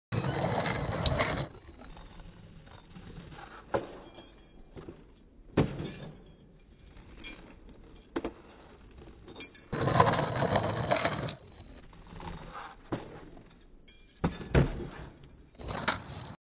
iPhone 5s Slow Motion Capture Skateboarding
Testing out my new iPhone 5s feature.